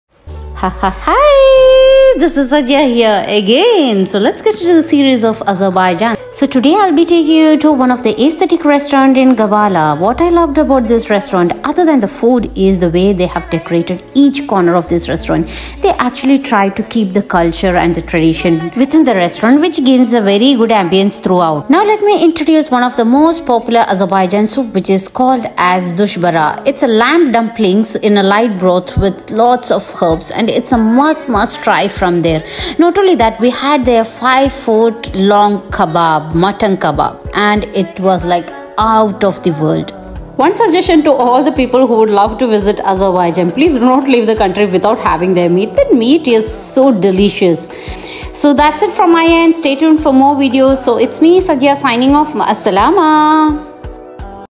Traditional Soup of Azerbaijan | Gabala |5ft Meat kabab |Azerbaijan series
Here goes one of the most popular soup of azerbaijan named dushbara ..This was delicious ..The best part anout this soup is less masala and yet flavourful..Very light on tummy ..I tried their meat kababa which was 5ft long..Its juicy ,meaty and delicious ..I tried both from a restaurant in Fontan restaurant in Gabala , Azerbaijan..Great ambience and good service too

Also dont forget to save & share for your future use